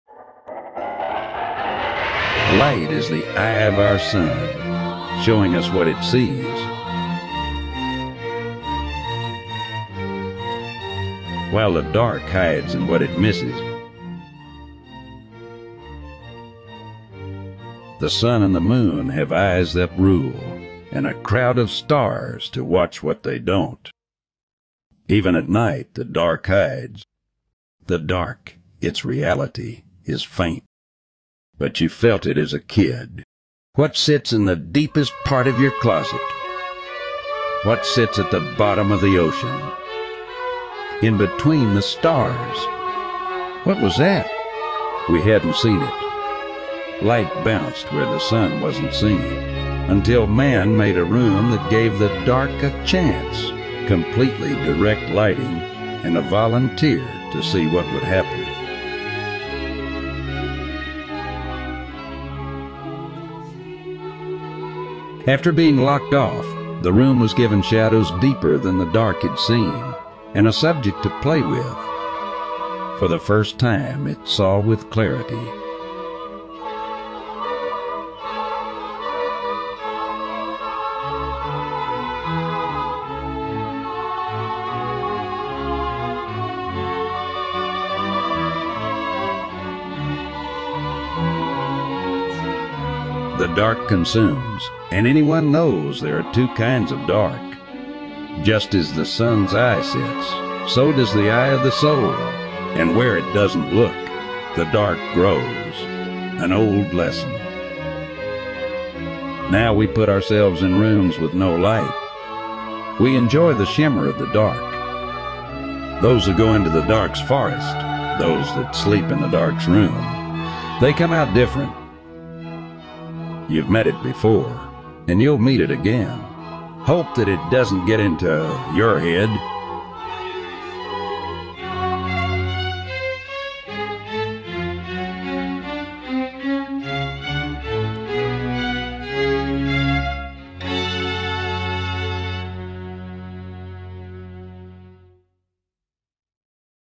The Shimmer of the Dark - Generative AI Horror
An experimental horror short film using generative AI animations as visuals over a short horror story written by me, narrated by an AI voice.
As a filmmaker, trying out new technology like this is both exciting and terrifying.
Made using Stable Diffusion and prompt engineering.